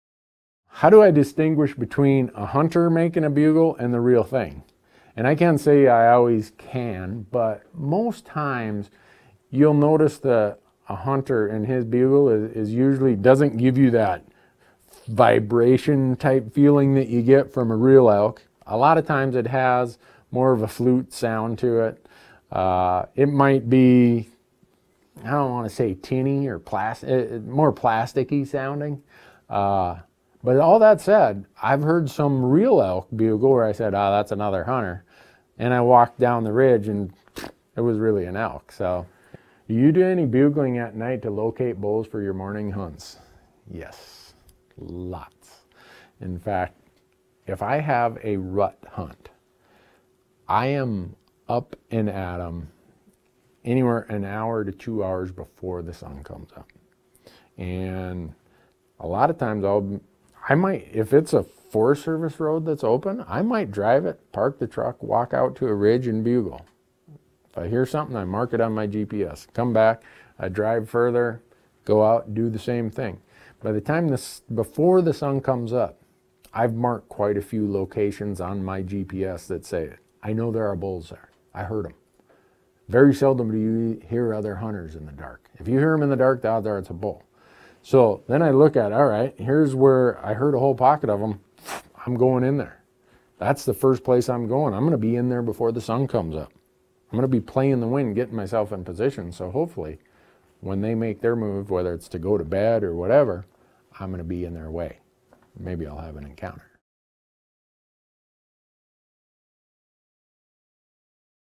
Elk Talk: Hunter Or Elk Bugle? / Locator Bugles At Night?
Randy shares his thoughts on how to determine if a bugle was a hunter or an elk.  He also discusses locator bugles at night.

Tune in Wednesdays at 7pP/10pE to ask your question.  Text the name RANDY to 77453 in US for an alert when Randy is live.